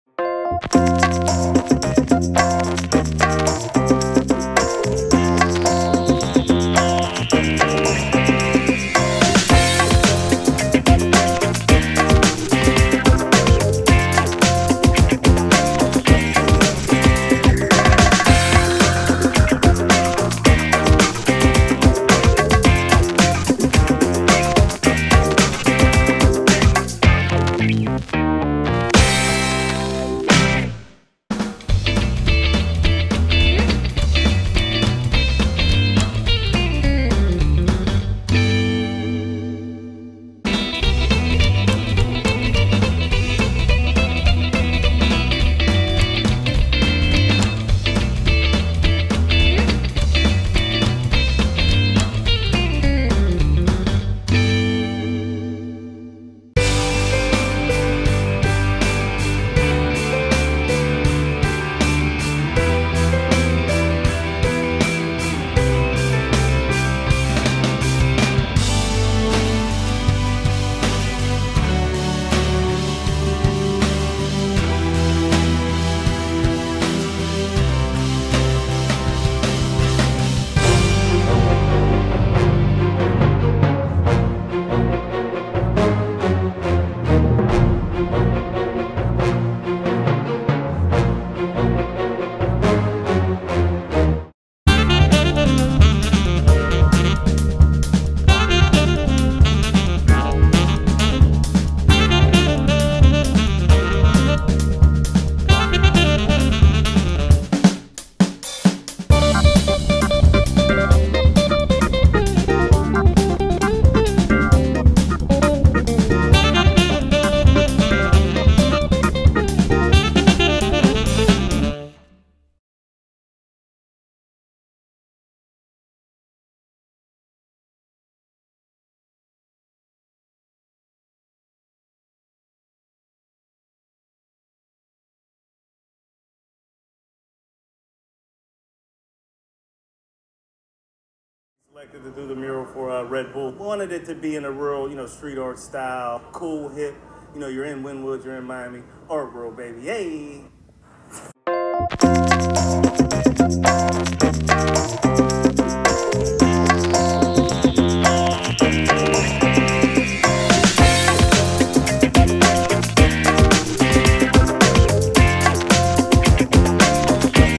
Miami Grand Prix Formula One Red Bull Racing Mural of Max Verstappen, Sergio Perez by Kyle Holbrook
Limited edition NFT available for Art Basel Miami 2022.

Print available in the Paddock only copyright Red Bull Oracle Racing.